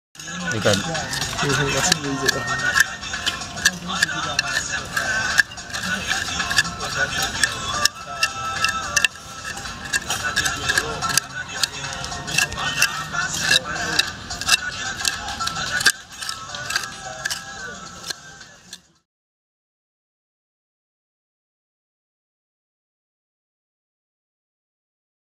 Is this another artefact at Christiansborg (Osu) Castle in Accra, Ghana?
The excavating team members must approach a possible artefact with caution in order to avoid damaging it. The chances are it is already highly frangible. Here, a trowel is used but if the artefact is smaller and more delicate, a brush will be used. After extraction it is carefully washed, bagged and labelled for further analysis later.